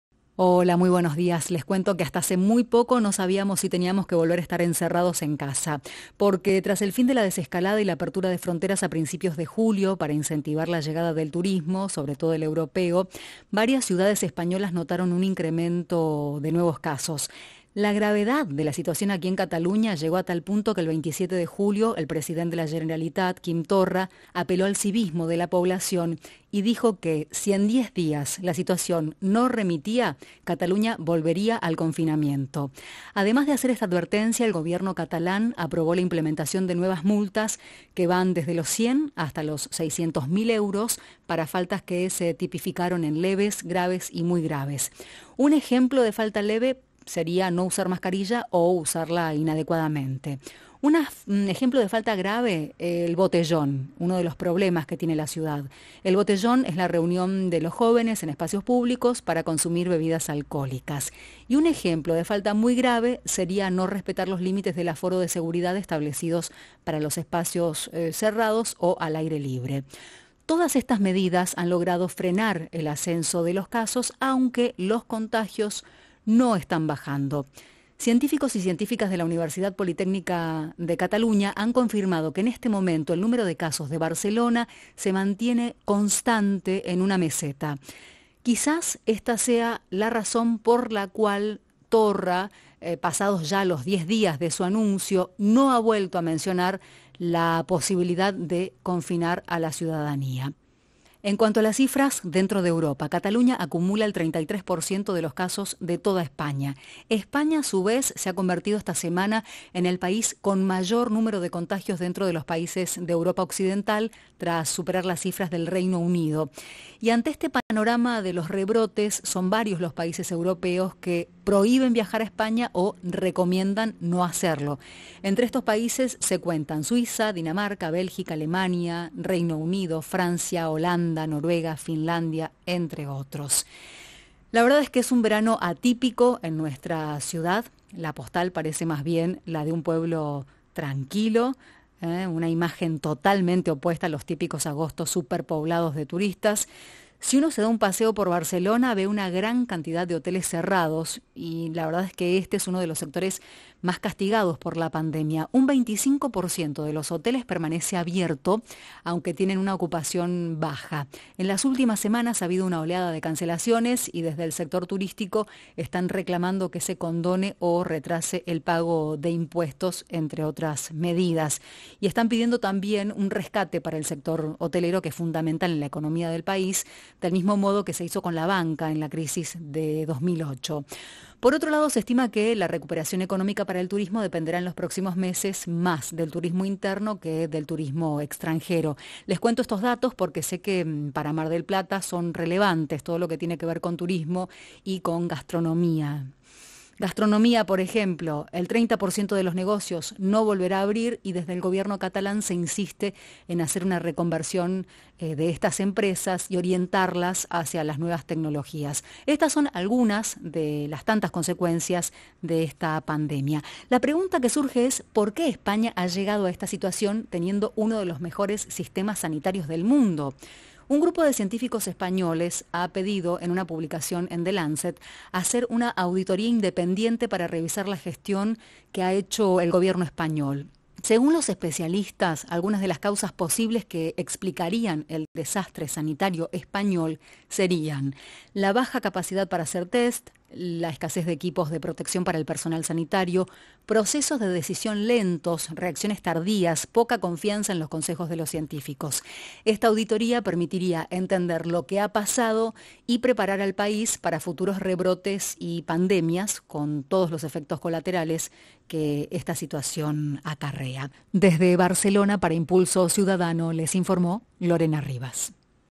España, primera en contagios
Informe para radio La Red, Mar del Plata. 8 de agosto, 2020.